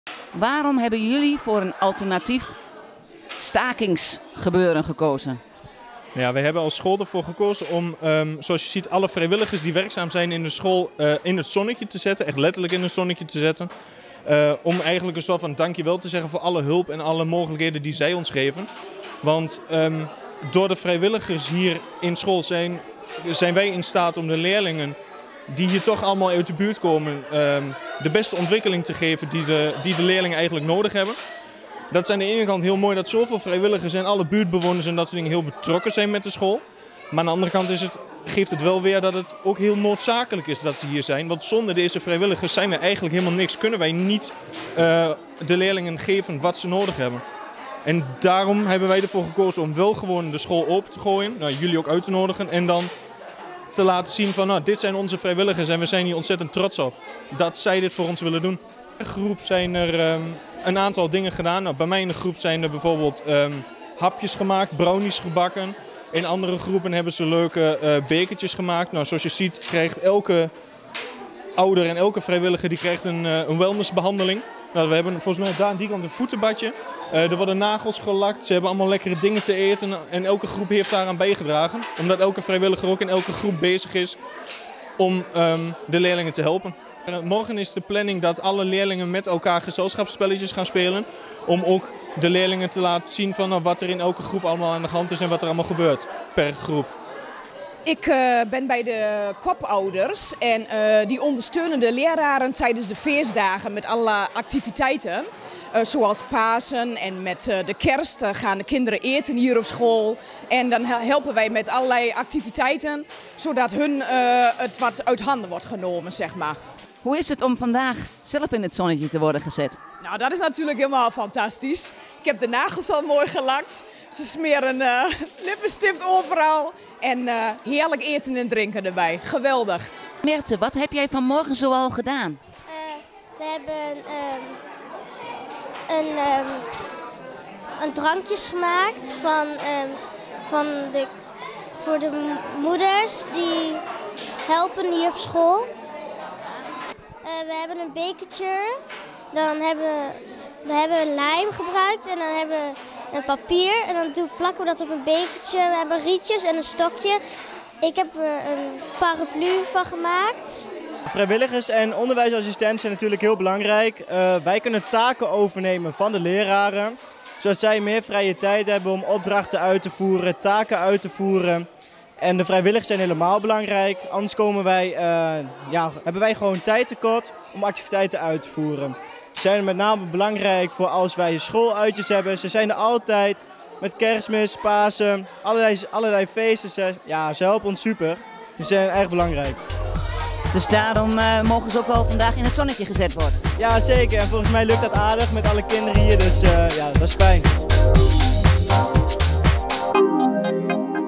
Staken, maar dan anders op de Stapvoorde
Op de landelijke stakingsdagen van het basisonderwijs werd er door de leerkrachten van de basisschool de Stapvoorde niet gestaakt, maar werden de ongeveer twintig vrijwilligers uitgebreid in het zonnetje gezet.